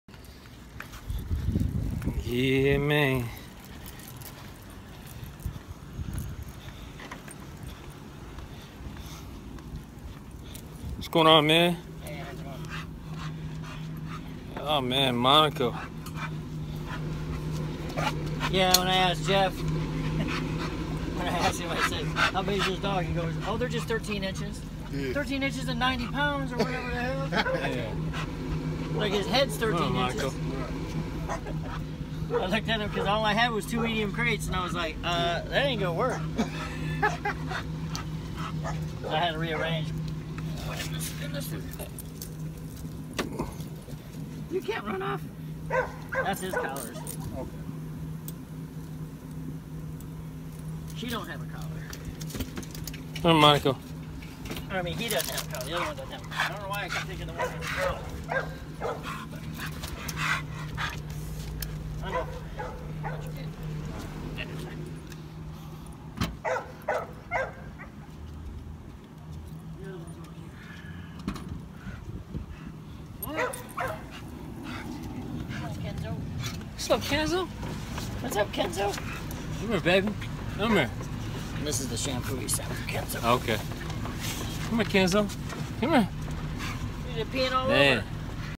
Mr Kenzo & Monaco arrived in Vegas
American Bullyz Mr Kenzo & Monaco arrived in Las Vegas.
Unafeyed Bullyz